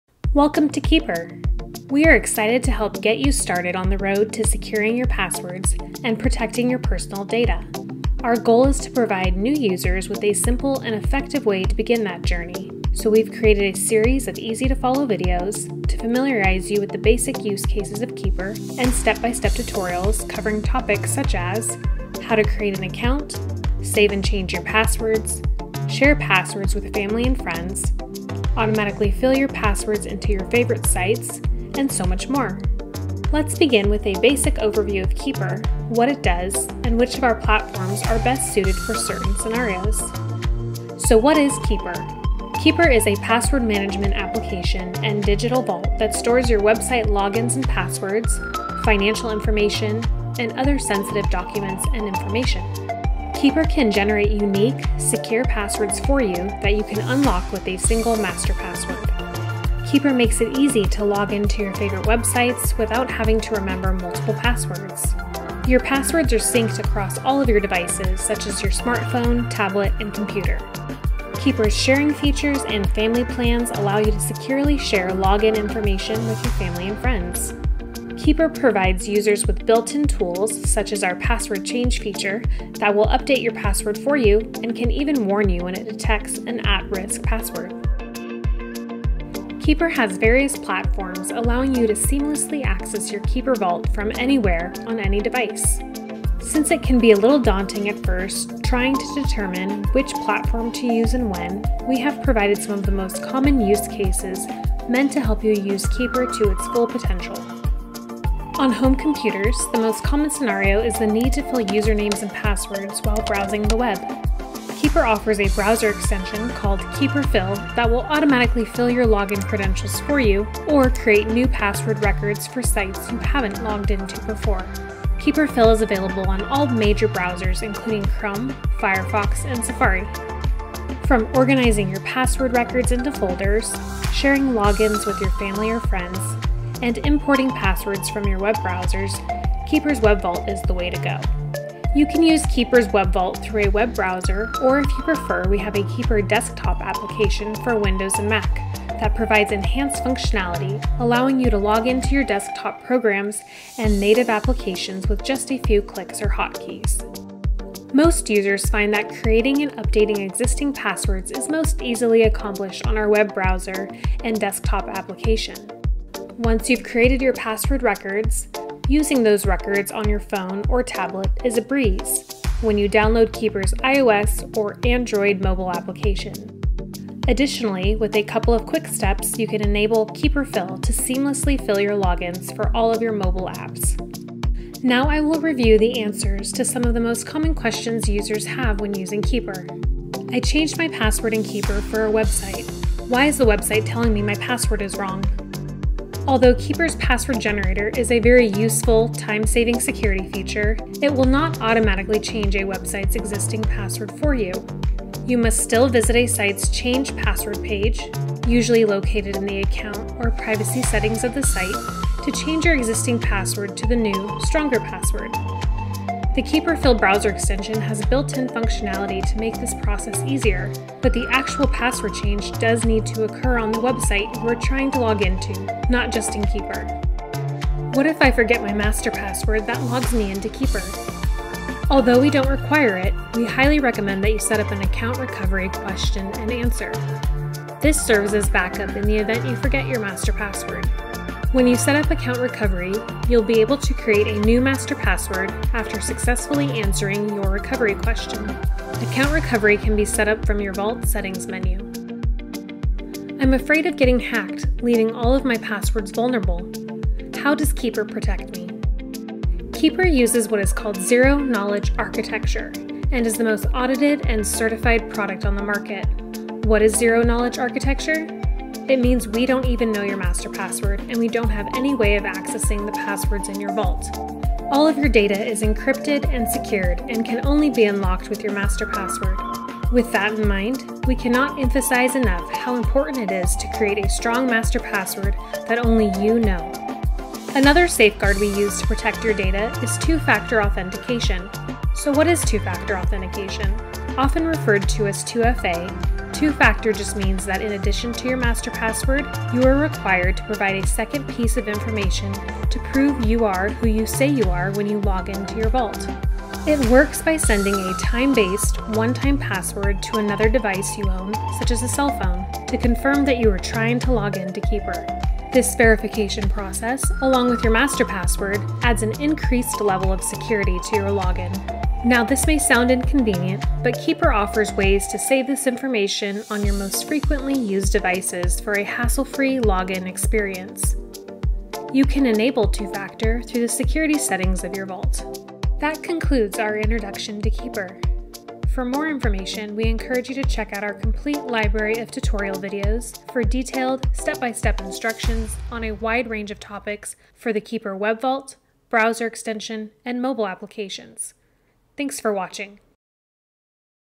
Welcome to Keeper
Welcome to Keeper! We are excited to help get you started on the road to securing your passwords and protecting your personal data. Our goal is to provide new users with a simple and effective way to begin that journey, so we’ve created a series of easy to follow videos to familiarize you with the basic use case of Keeper and step-by-step tutorials covering topics such as: how to create an account, save and change your passwords, share passwords with family and friends, automatically fill your passwords into your favorite sites and so much more!